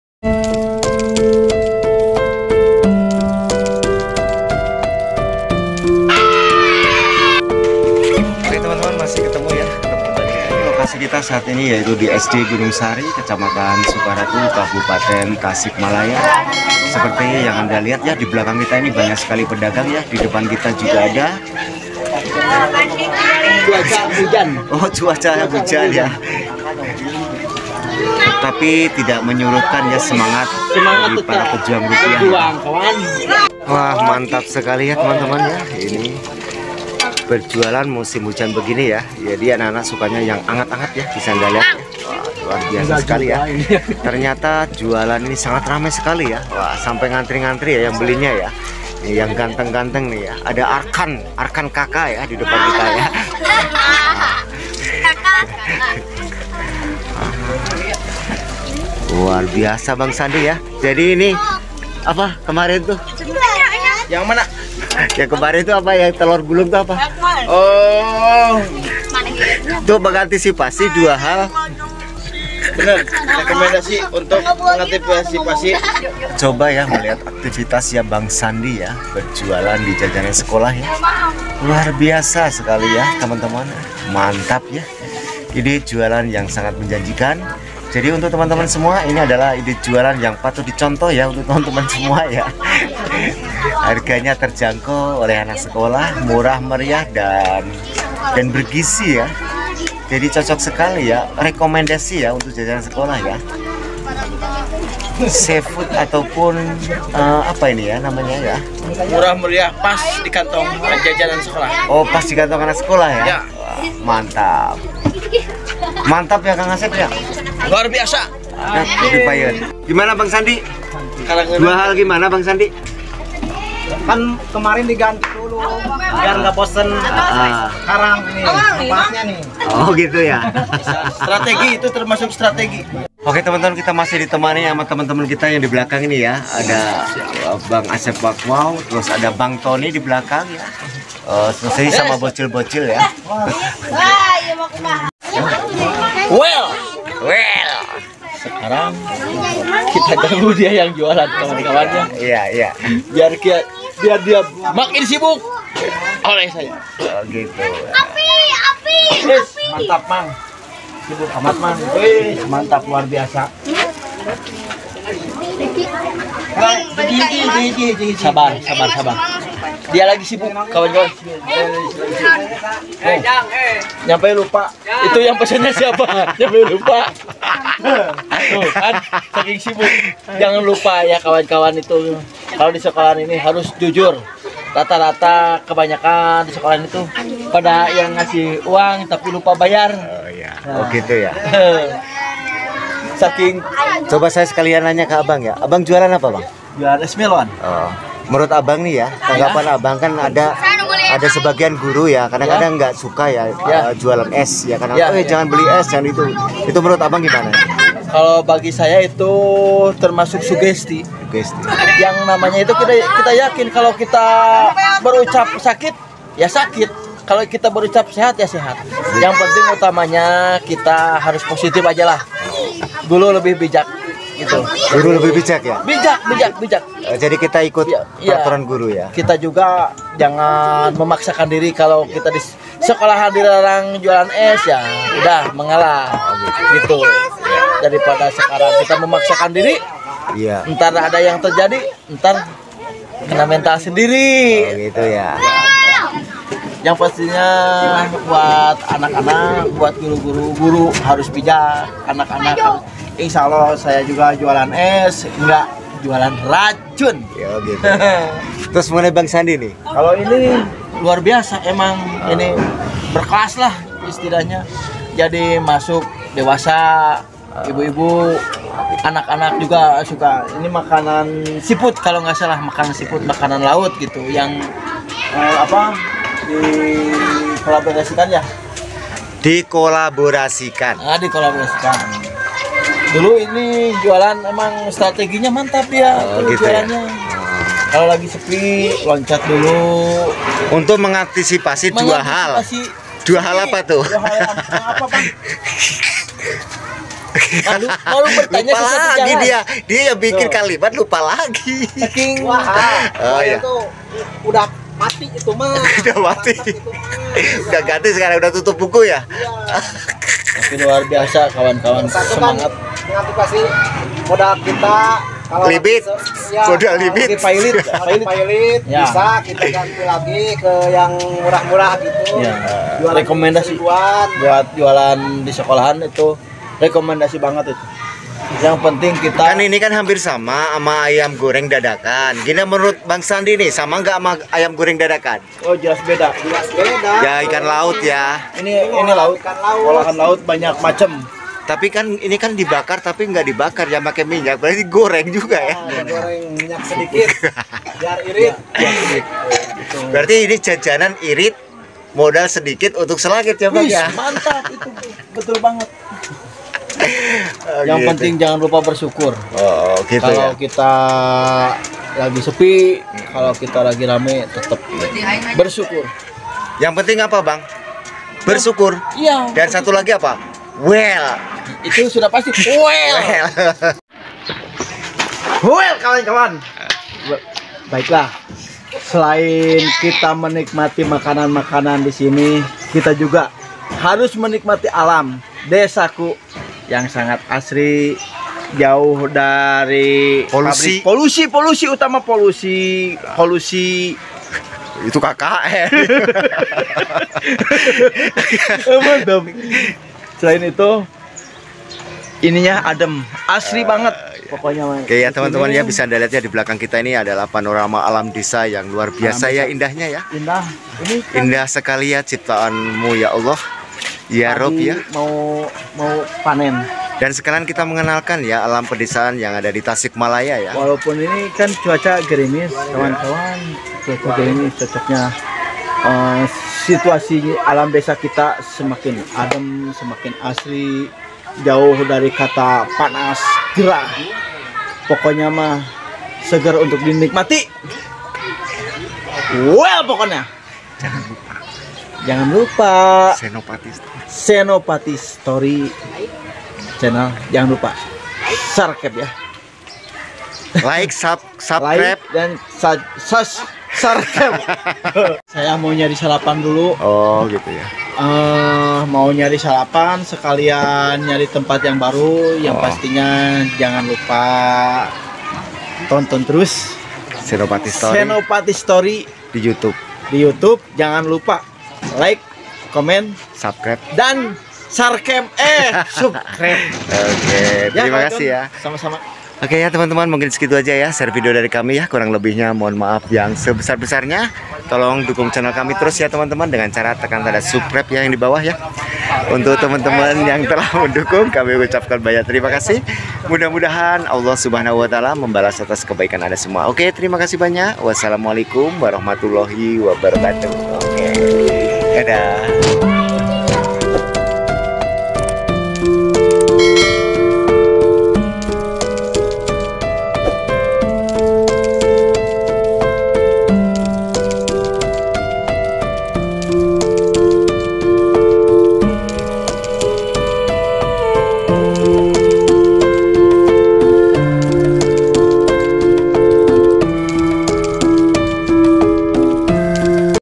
ide jualan viral 2023 sangat laris tidak ribet dan tidak mengenal cuaca ..
ide jualan yang menjanjikan
serta bisa bercanda dengan pedagang lainnya